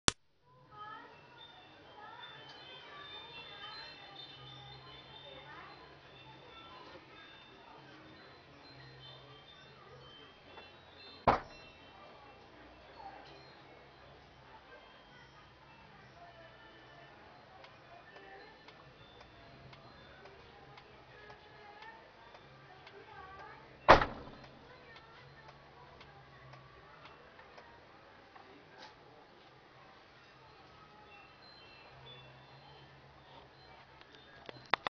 Daihatsu Charade Domelight Fader (day mode)
10sec fading effect on domelight.